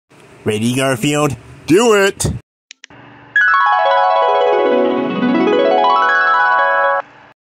Garfield Comic Dub for December 18th, 2023
Garfield and Jon get to see the Christmas tree light up!

Garfield is owned by Jim Davis, PAWS Incorporated, Nickelodeon, and Paramount Global.